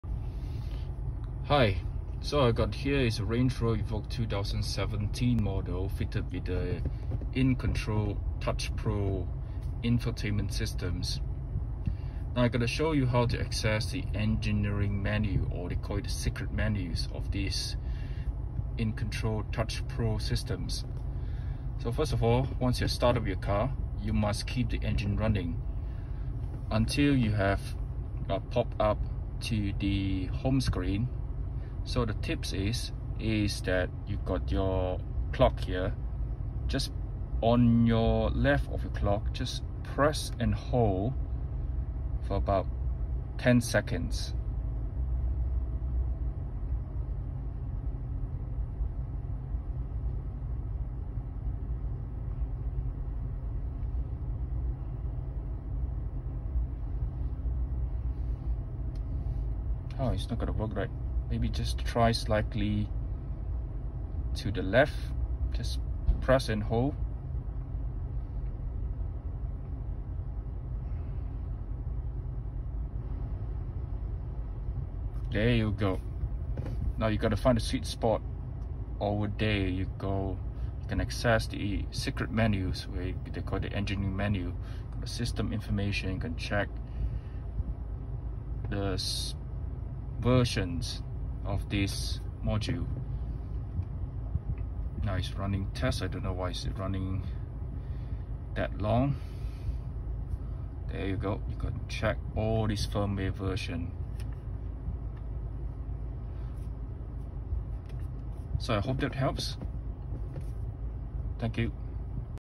Engineering Menu (Secret Menu) - InControl Touch Pro Range Rover Evoque 2017
A short clip on accessing the engineering menu aka secret menu on InControl Touch Pro fitted with Range Rover Evoque 2017MY.